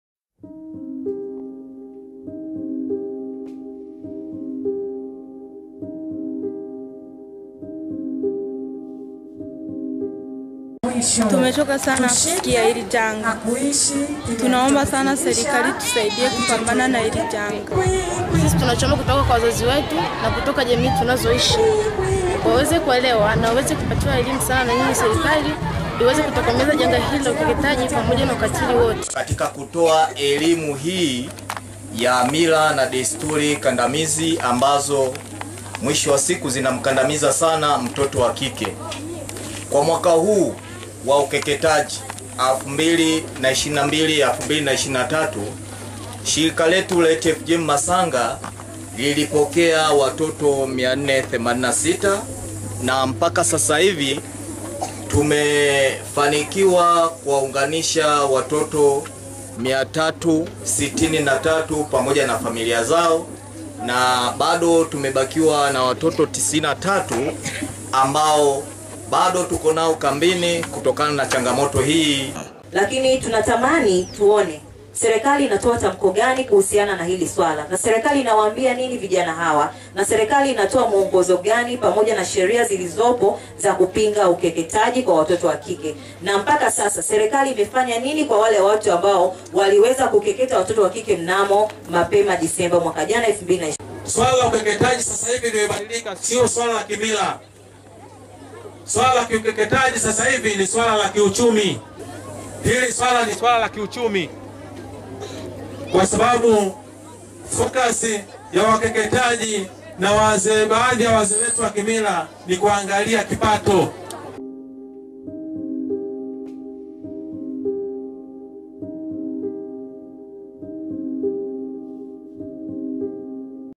WASICHANA WACHOSHWA NA UKEKETAJI WAOMBA SERIKALI IWASAIDIE
Zaidi ya wasichana 480 walikimbilia kituo cha Masanga ili wasikeketwe